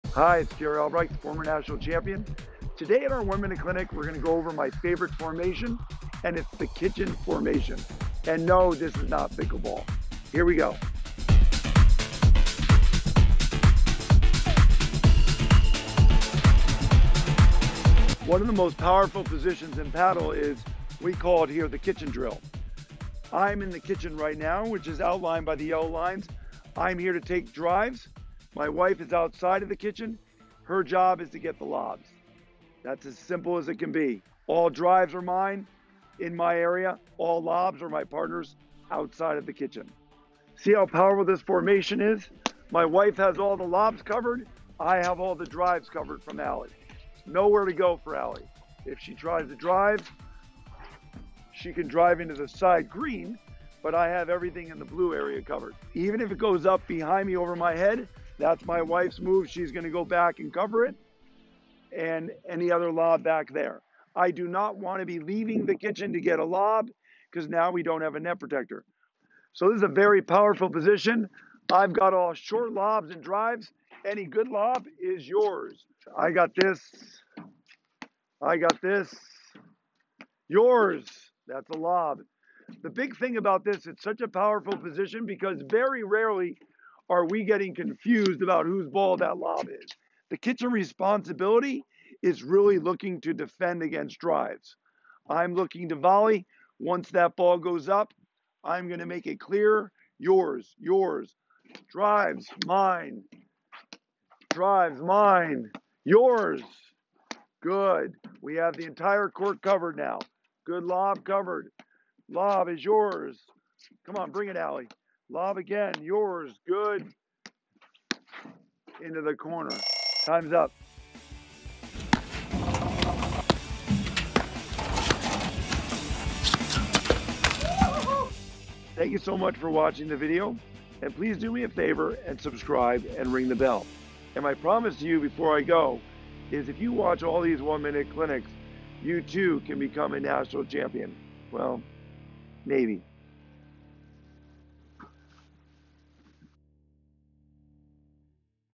One-Minute Paddle — Court Positioning: The Kitchen Formation
National paddle champion Jerry Albrikes shares his secret court formation for winning in platform tennis.